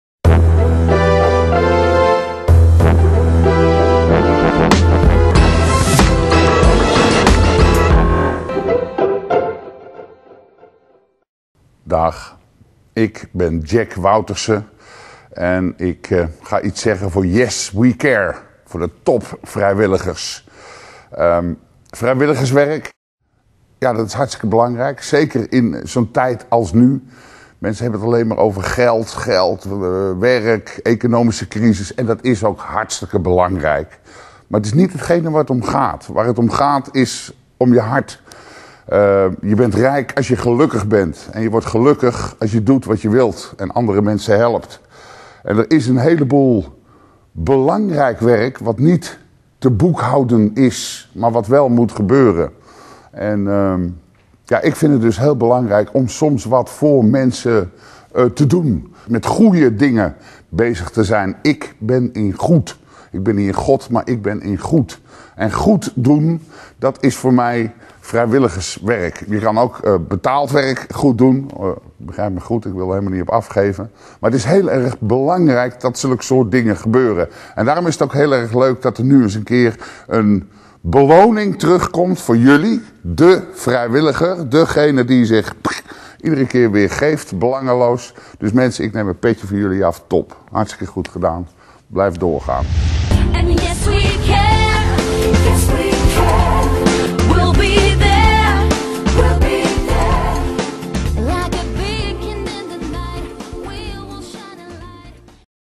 Jack Wouterse
Jack Wouterse persoonlijke videoboodschap voor de TopVrijwilligersdag, vanuit het Ro Theater, speciaal voor Yes We Care.